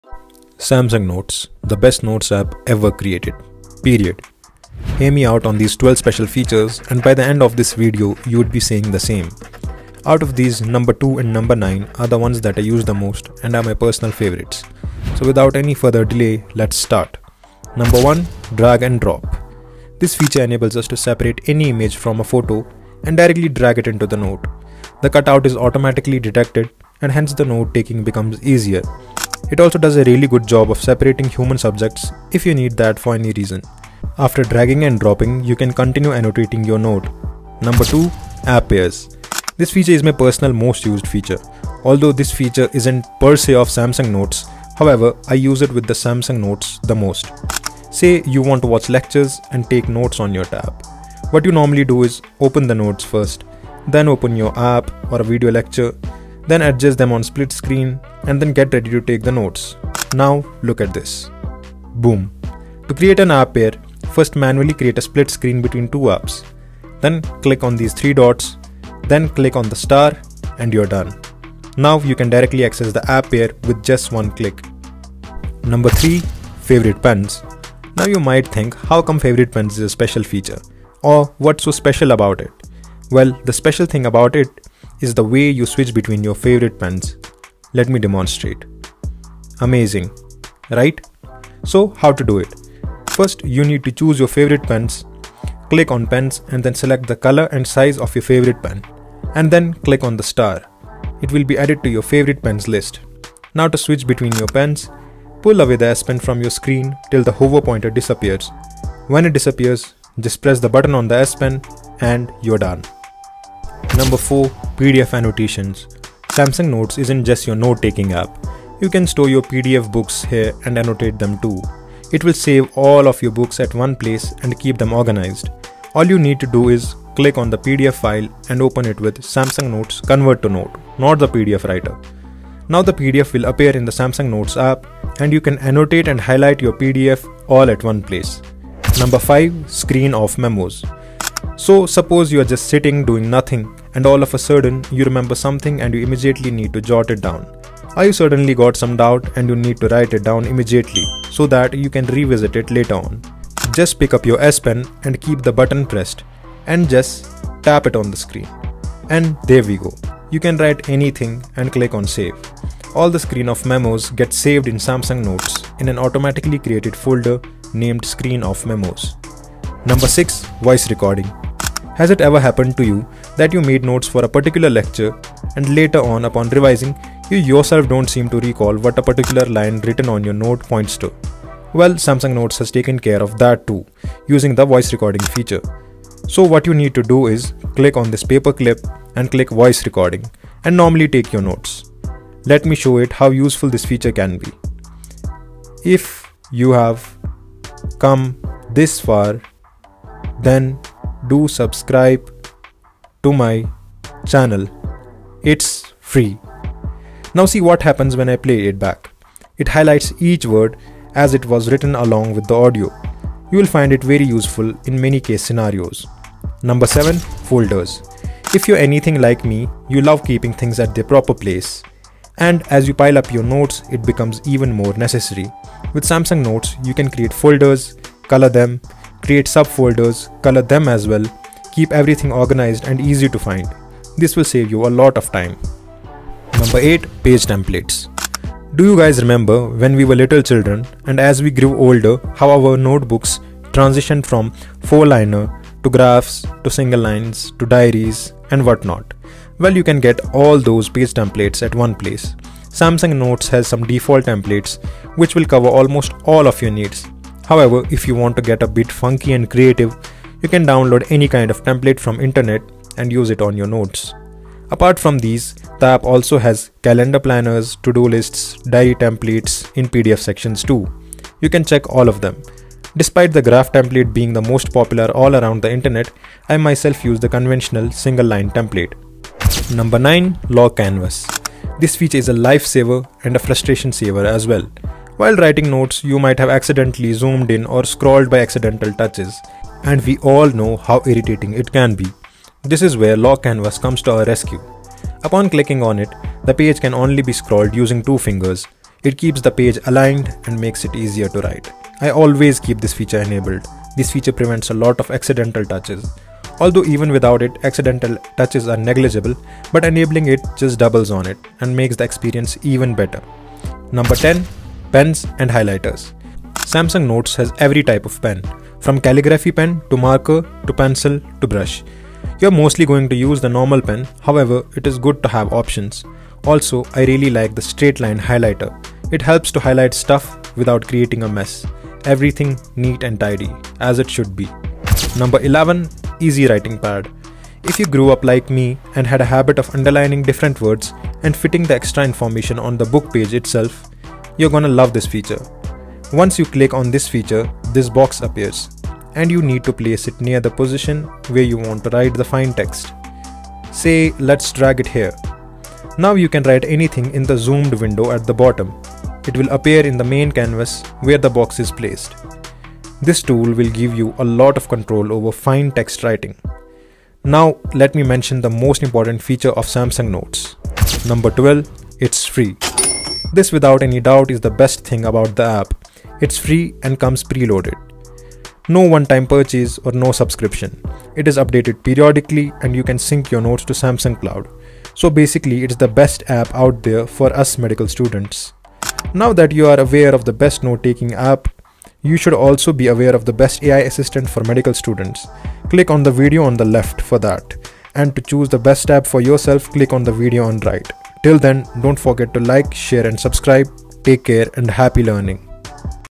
THE SAMSUNG NOTES HACK EVERY MEDICAL STUDENT WISHES THEY KNEW EARLIER
Samsung notes is the best note taking app ever created and join me as i show you amazing 12 features of this note taking app.
This app works on all samsung galaxy tabs and is free to use. This app is a treasure for Medical students pursuing their MBBS.
it will prove to be the best productivity app for MBBS students. and you can take all notes on your app be it anatomy, physiology, biochemistry. pharmacology, pathology, microbiology or anything for that matter.

In this video, we dive into the incredible features of the Samsung Notes app that can transform the way medical students take notes and organize their study materials. Whether you're in lectures, studying for exams, or preparing for clinical rotations, these tips will help you maximize your productivity and efficiency.

✨ What You'll Learn:
- How to create and organize notes effectively
-Anuj Pachhel productivity
- Tips for using handwriting and drawing tools
- Ways to integrate multimedia into your notes

Join me as we explore the 12 best features of Samsung Notes that every medical student should know.

time stamps:
00:00 Introduction
00:17 Drag and Drop
00:37 App pairs
01:16 Favorite Pens
01:54 Pdf annotations
02:19 Screen Off Memos
03:37 Folders
03:57 Page templates
04:40 Lock canvas
05:14 Highlighters and pens
05:36 Easy writing Pad
06:11 It's Free